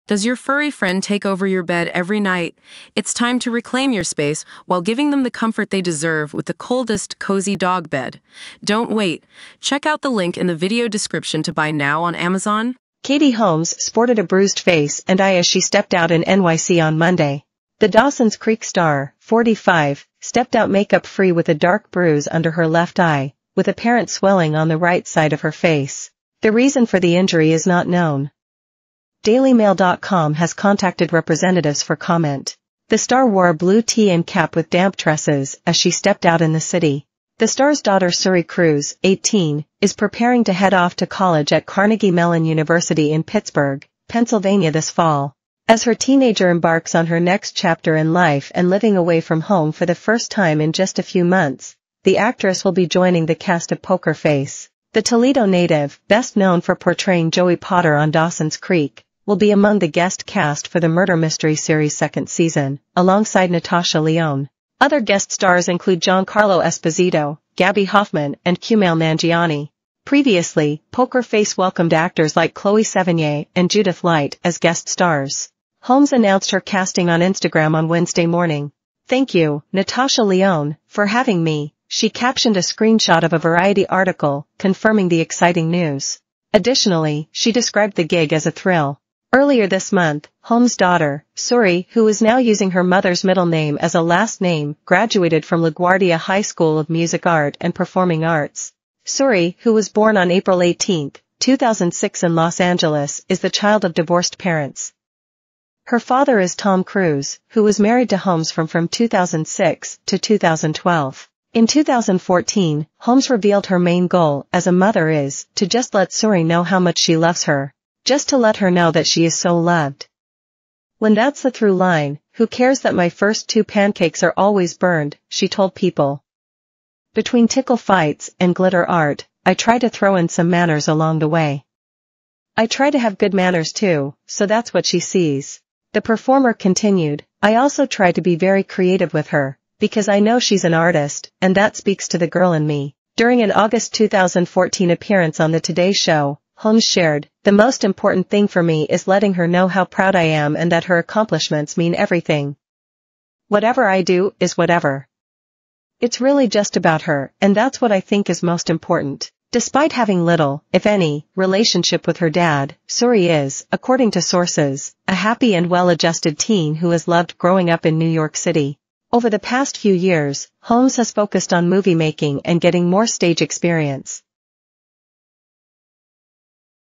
Katie Holmes shows off a bruised face and eyes while keeping a low profile under a baseball cap in
Transform your pet's sleep experience with the Coldest Cozy Dog Bed
Katie Holmes shows off a bruised face and eyes while keeping a low profile under a baseball cap in New York, as her daughter Suri Cruise prepares to leave home for college.